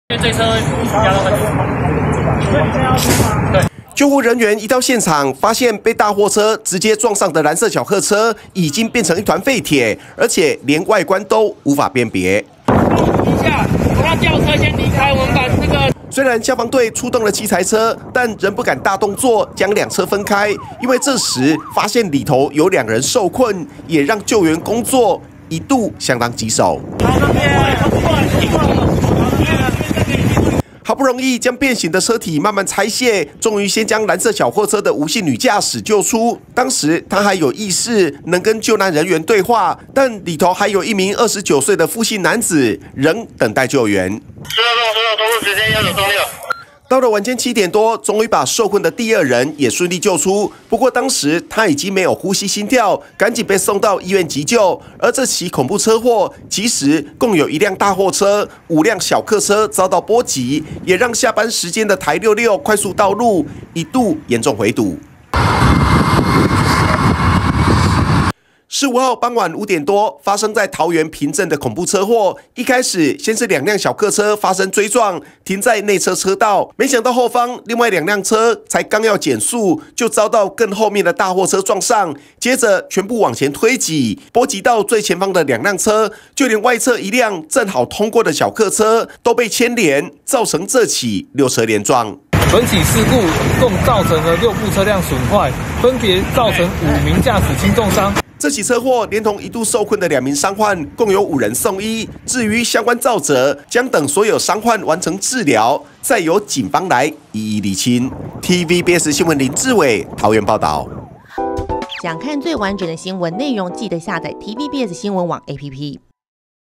大貨車釀六車連撞 1車成廢鐵2人1度受困 台66大回堵｜TVBS新聞
台66快速道路的桃園平鎮段，15號傍晚五點多，發生一起大貨車跟小客車的恐怖六車連撞意外，共造成五人受傷，其中一輛被大貨車直接撞上的小客車，幾乎潰縮成一團廢鐵，還造成兩人一度受困，直到晚間七點多，才將兩人救出送醫。